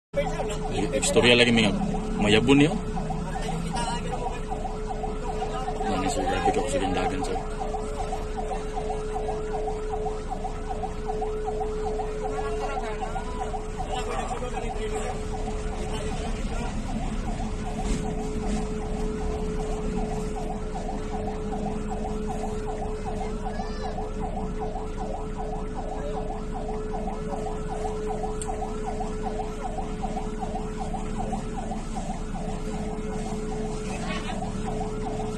Aksidente sa Sultan Naga Dimaporo, Lanao del Norte
Mao ni ang nahitabo kaganihang alas 3 June 4, 2023. Nagbangga ang usa ka trailer ug bonggo didto sa may SND, LDN. Makita usab sa video ang kuha sa dash cam gikan sa gasunod nga sakyanan sa trailer. Base sa atong nakuha nga report DOA ang duha ka bata nga sakay sa bonggo. Paaboton pa nato ang uban pang detalye.
(97.10 Spring FM News Department)

Like, Follow and Share our Facebook Page. Listen to us also at 97.10mhz on your FM Radio.